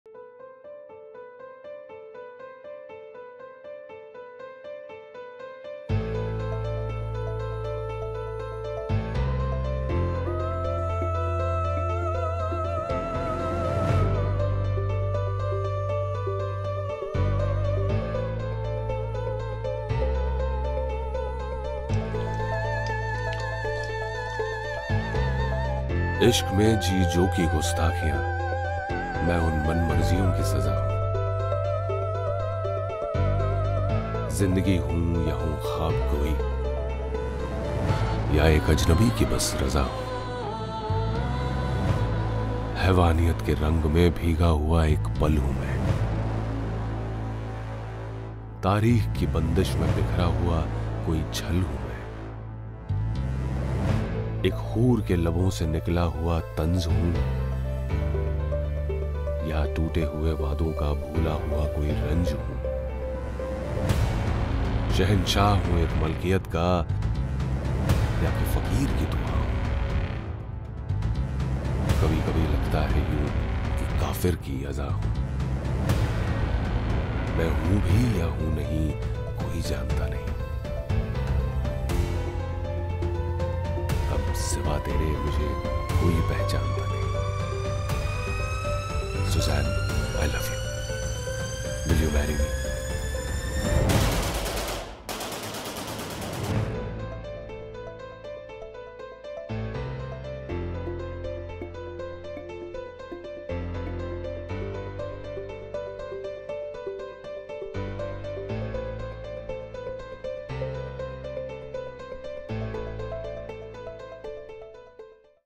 AI Movie Trailer Maharaja in Denims
🎬 Welcome to a groundbreaking moment in cinematic history! Behold the first-ever movie trailer entirely crafted by AI, bringing to life the beloved world of the 2014 bestseller "Maharaja in Denims."

Prepare to be mesmerized as AI technology weaves together stunning visuals and captivating storytelling to offer a glimpse into this fantastical world. The trailer is a testament to the power of artificial intelligence in unleashing creativity and redefining the boundaries of filmmaking.

Join us on an unforgettable journey as the AI brings to life the vivid characters, immersive landscapes, and gripping narrative from the pages of the acclaimed novel. Get ready to witness a fusion of cutting-edge technology and timeless storytelling that promises to leave you spellbound.

Experience the magic of AI-driven creativity and get a sneak peek into the cinematic universe of "Maharaja in Denims." Don't miss this historic moment in entertainment – watch the trailer now and immerse yourself in a new era of filmmaking! ✨🎥